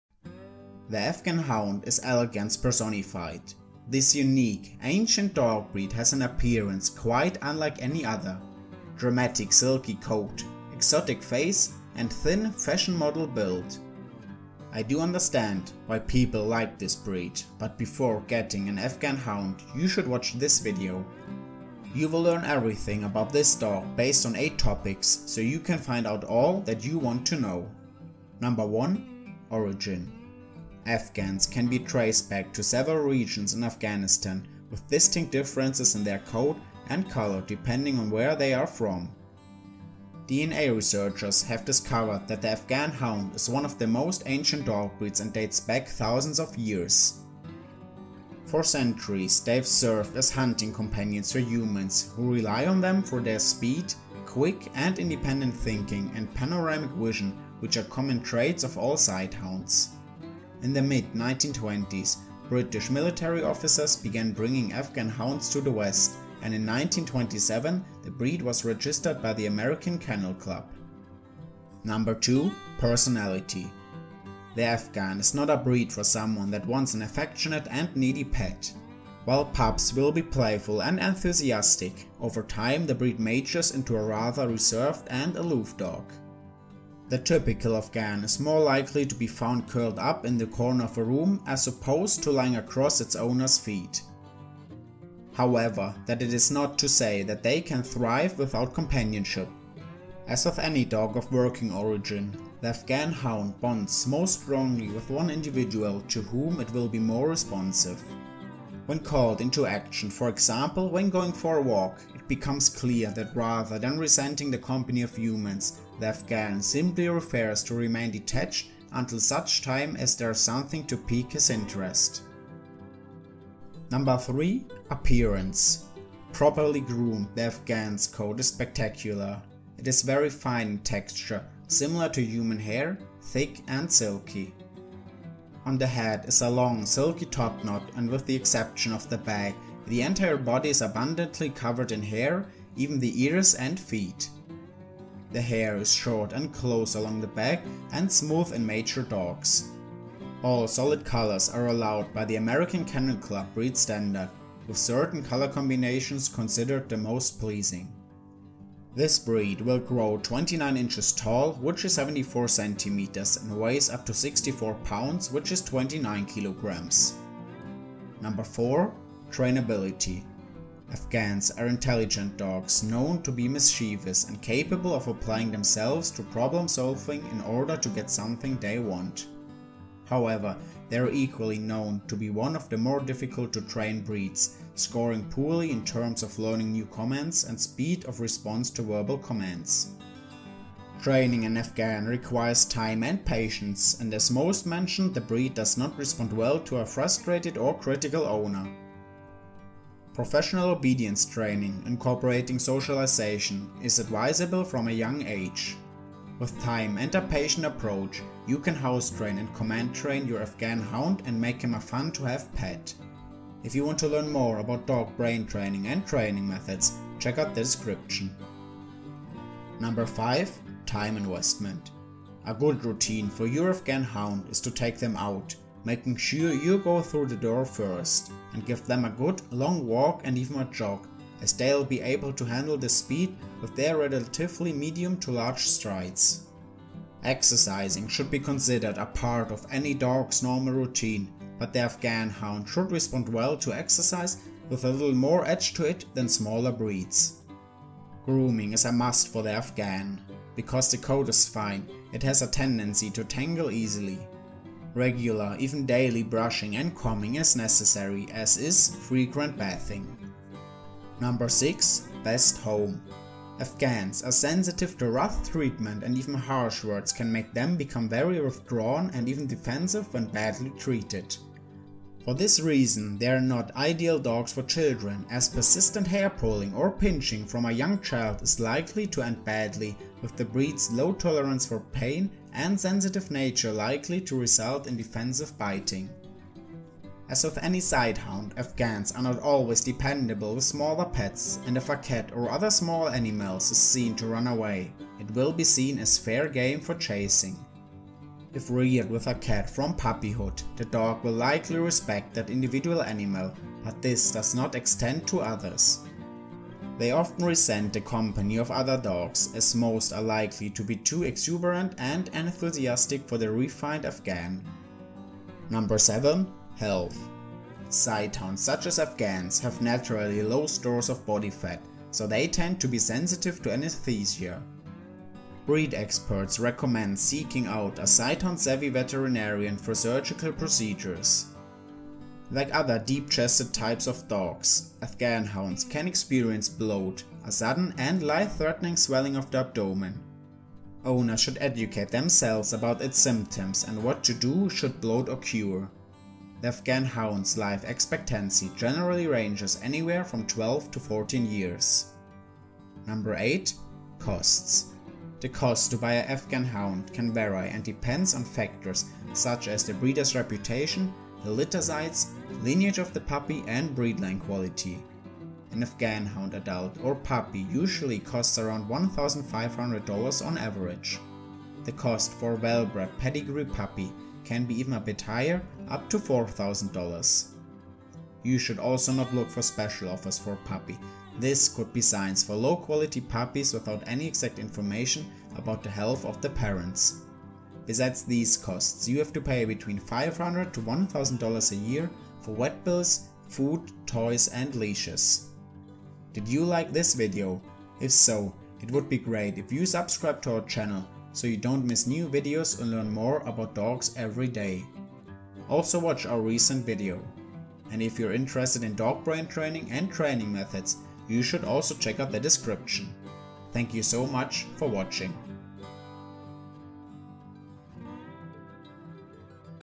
Afghan Hound - Dog Breed Information
Everything you need to know about the Afghan Hound Dog Breed.

The Afghan Hound is elegance personified. This unique, ancient dog breed has an appearance quite unlike any other: dramatic silky coat, exotic face, and thin, fashion-model build. I do understand why people like this breed, but before getting an Afghan Hound you should watch this video. You will learn everything about this dog based on 8 topics, so you can find out all that you want to know.